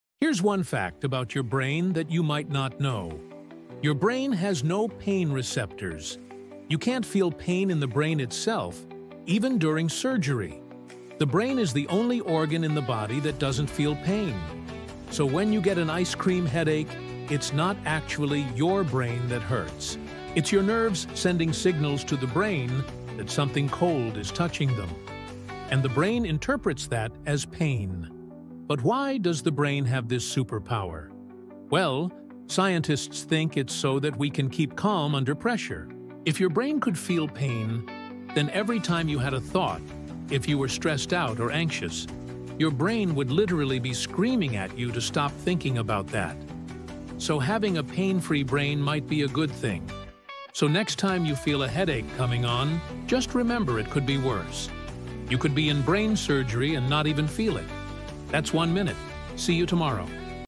Your Brain Can’t Feel Pain… Ever 😳🧠
Your brain controls everything you feel — but it can’t feel pain itself! 😳 That’s because it has zero pain receptors. This strange fact is why doctors can perform brain surgery while patients are awake. Mind = blown.